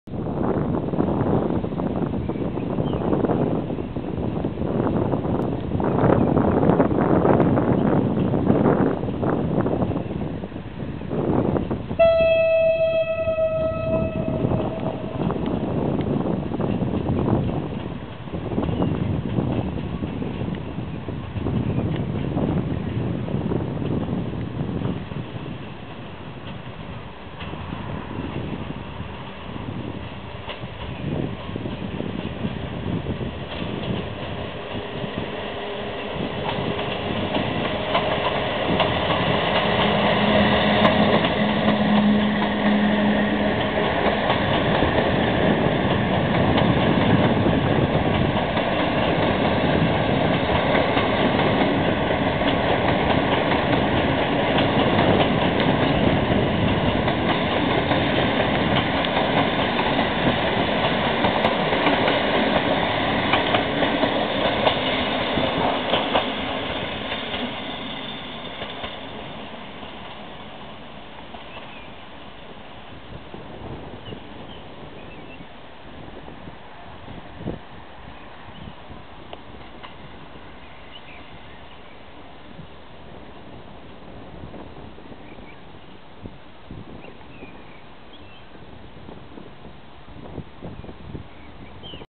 |27.03.2012|EU07-067|TLK83202 "Ustronie"|Wyjazd z Kołobrzegu + Baczność|
Spokojny wyjazd siódemki z "Ustroniem" do Krakowa, przy okazji mech zapodał RP1, za co dzięki.